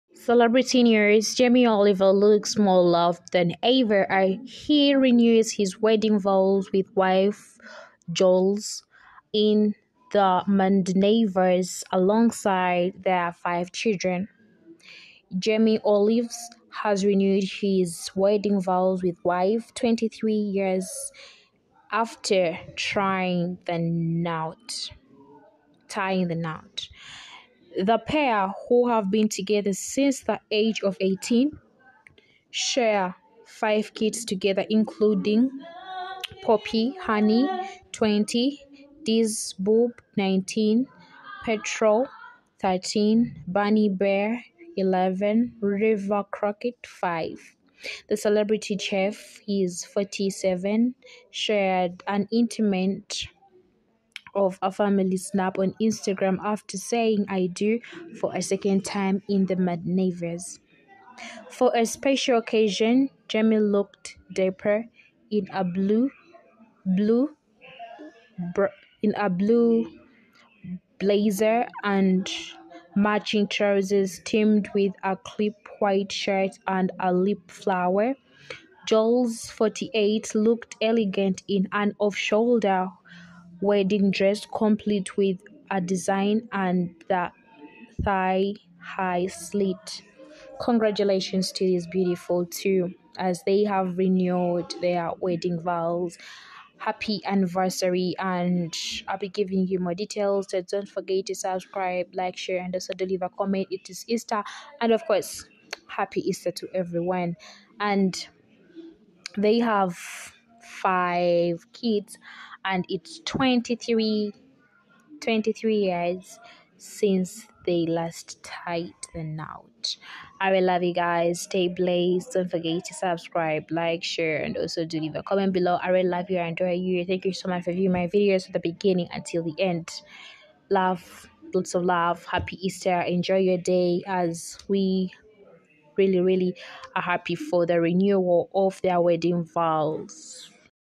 Jamie Oliver & Jools Renews Wedding Vows | In The Maldives Alongside Their Five Children
Jamie Oliver looks more loved-up than ever as he renews his wedding vows with wife Jools in the Maldives alongside their five children